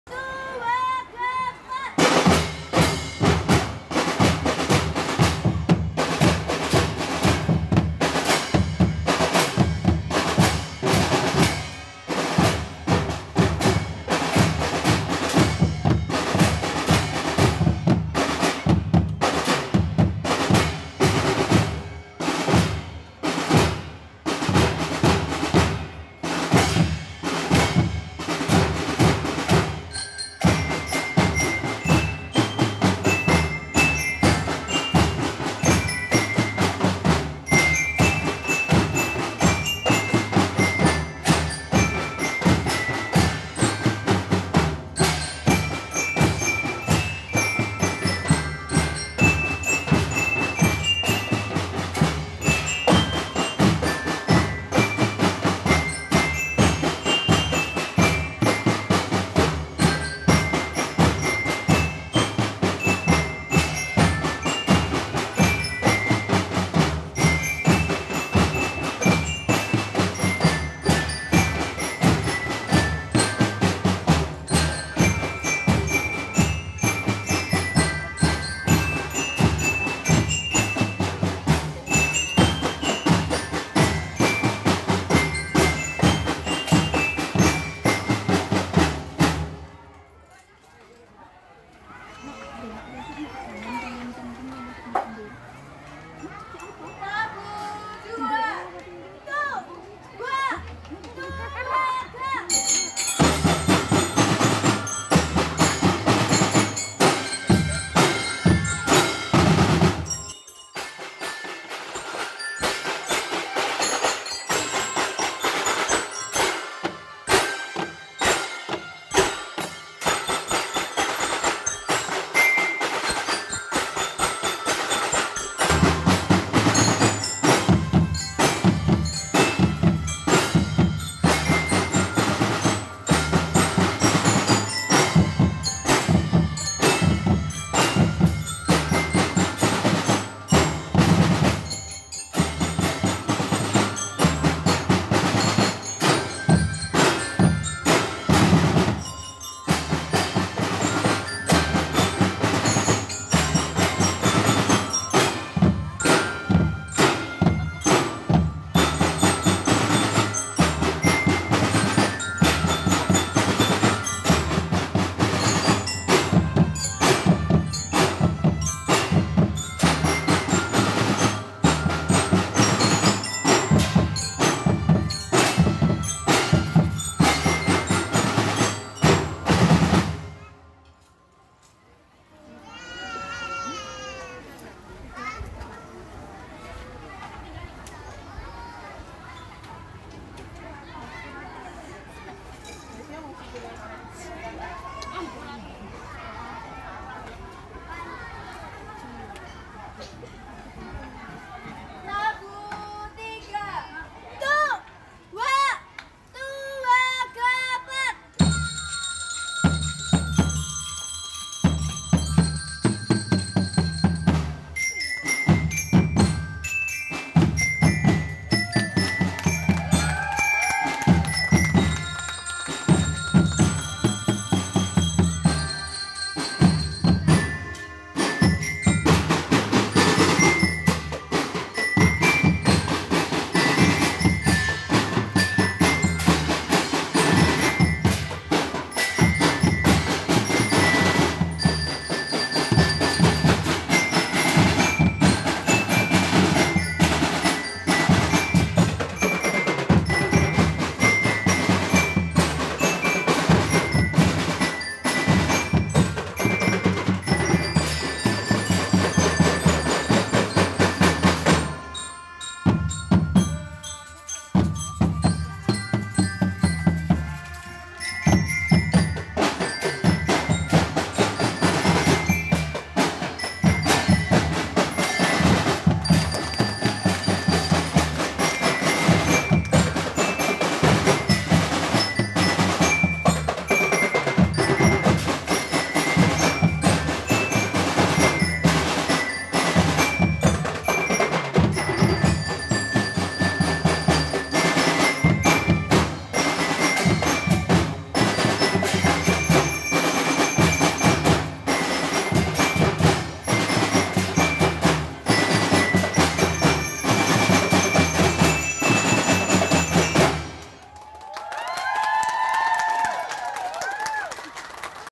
Drumband SD N Ringinharjo Jogja Marching Festival Waterpark Gabusan 2023
Jogja Marching Festival 2023, lomba drumband Antar TK dan SD se Provinsi DIY Bertempat di waterpark Gabusan Bantul Yogyakarta.